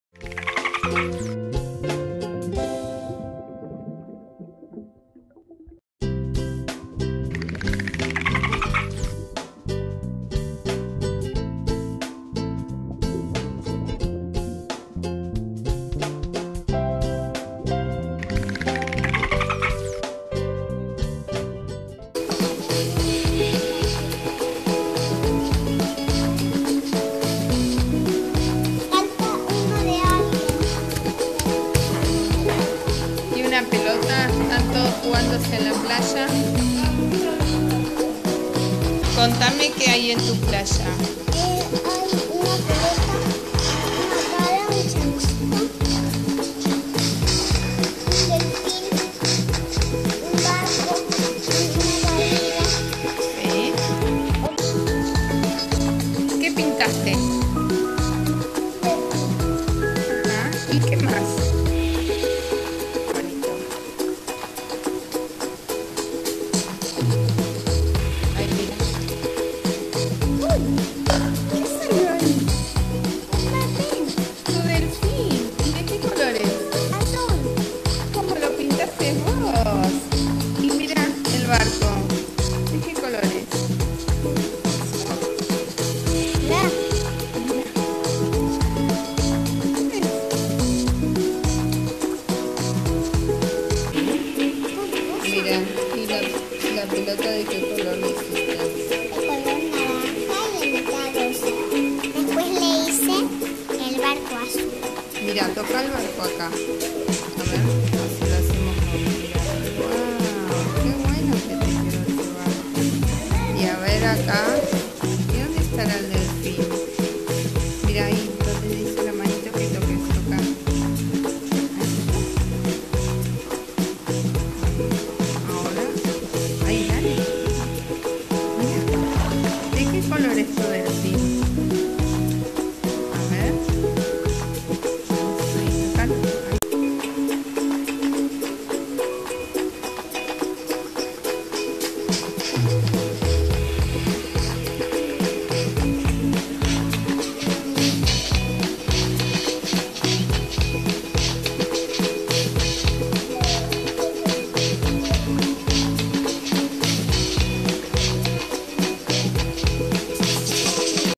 Ahí viene la ola - Act. de Realidad Aumentada
Después de leer el relato "Ahí viene la ola", del libro "Milo tiene coronita" de Moria Ramos, animamos una playa en papel pintada por los nenes.

Sala de 4 años -Jardín Newlands
App Quiver